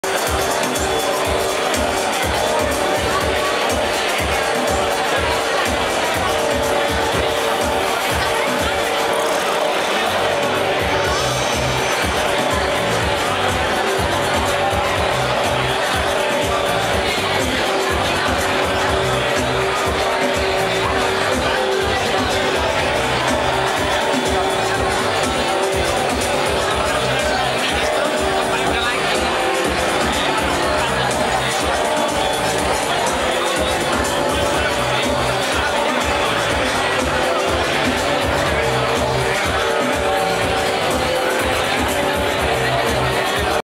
XBOX 360 Live
Fiesta XBOX 360 live. Para presentar juegos y novedades en la consola. Madrid